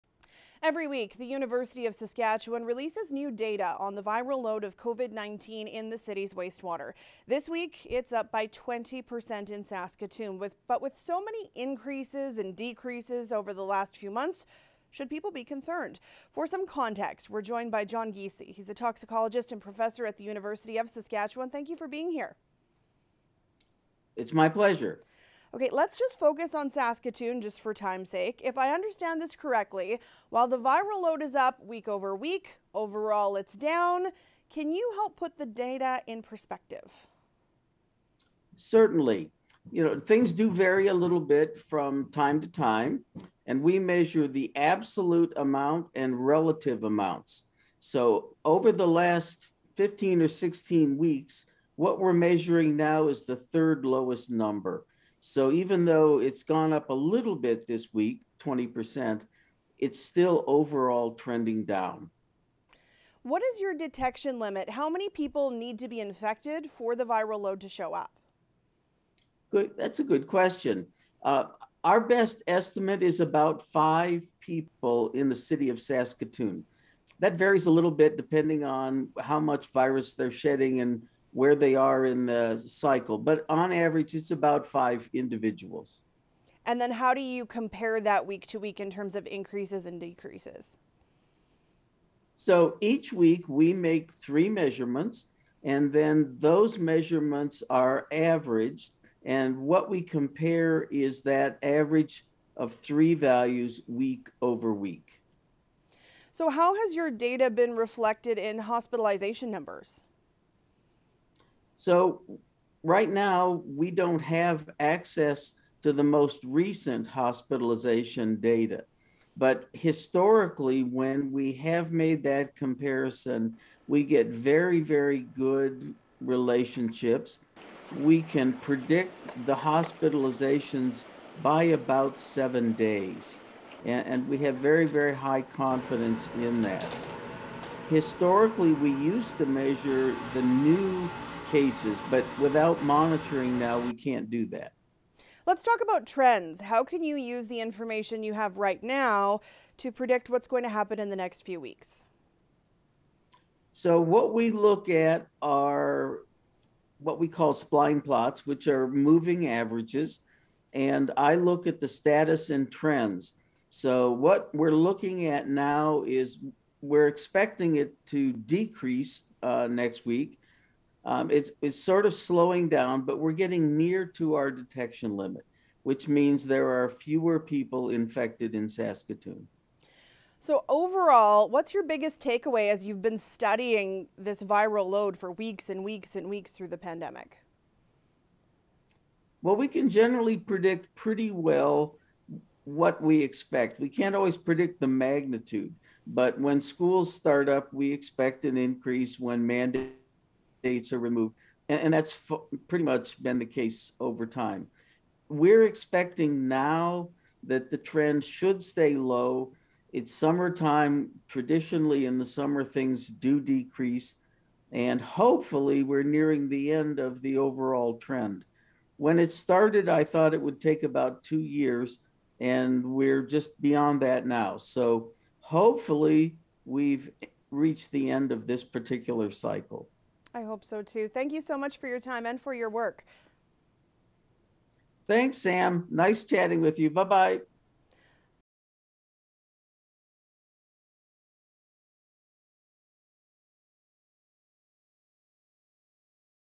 A closer look at what the wastewater research numbers mean for COVID-19 cases in Saskatchewan
University of Saskatchewan toxicologist John Giesy breaks down what the trend of wastewater COVID-19 data means for what we can expect in Saskatoon.